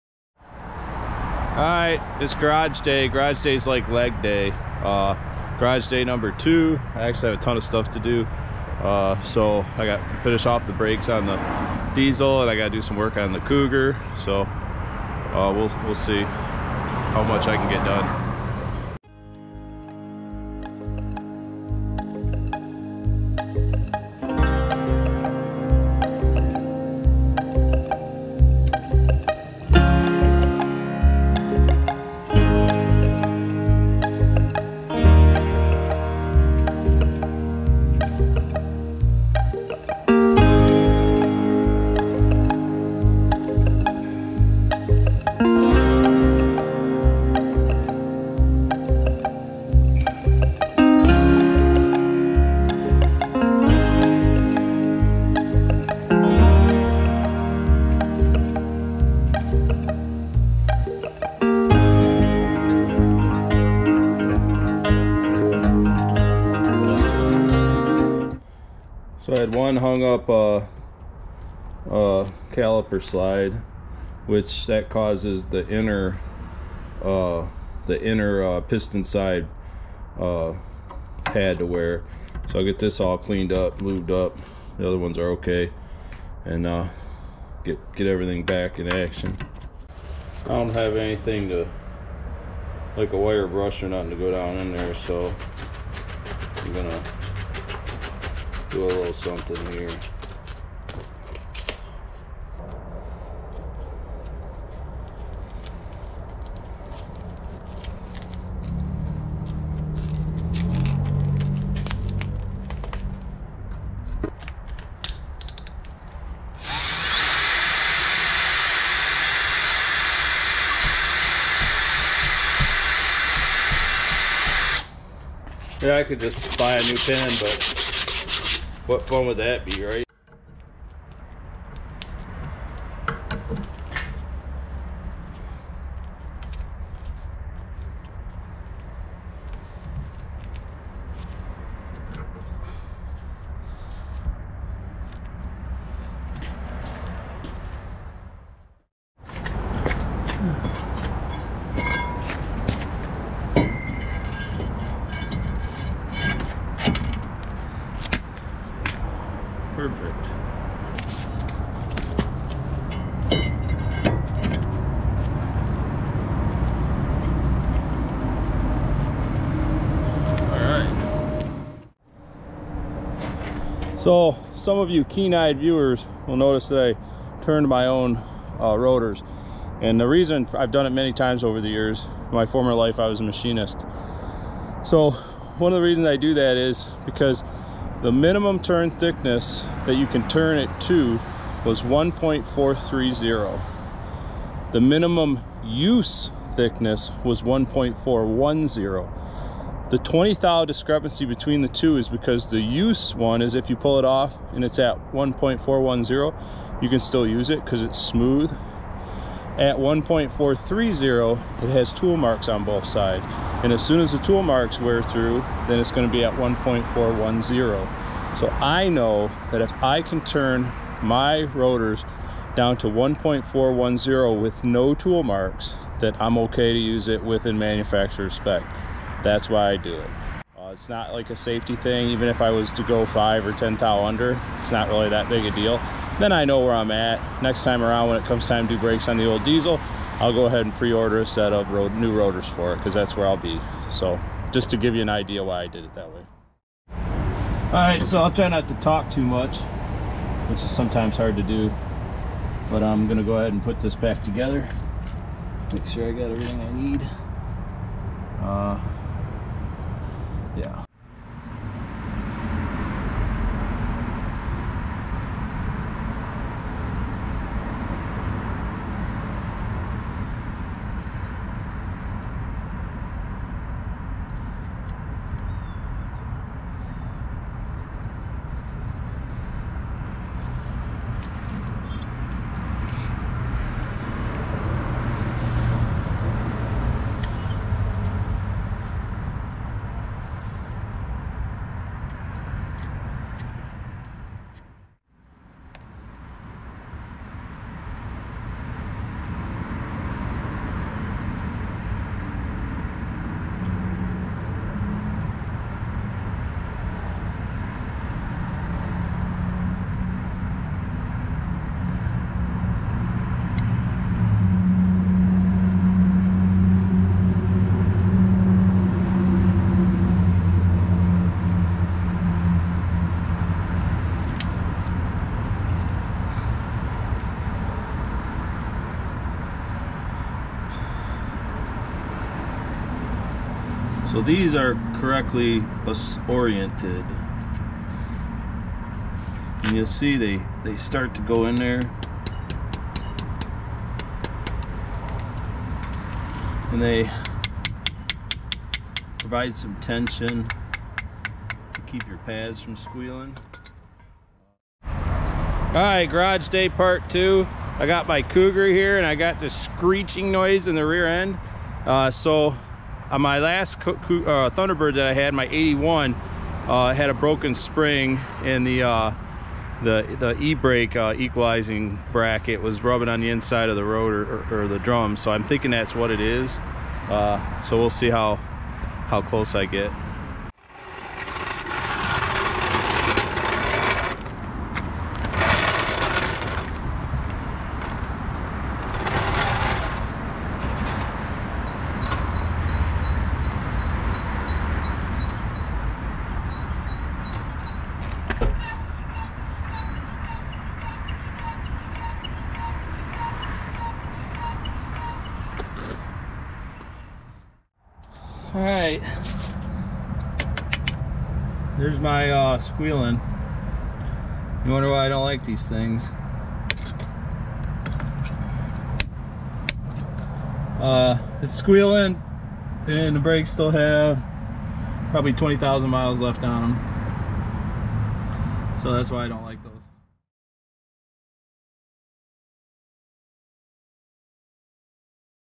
F-350 front brakes, can I turn the rotors????
I spent the day working on my F-350 and I might have even turned my rotors myself. finally got my cougar to stop making a screaching noise also.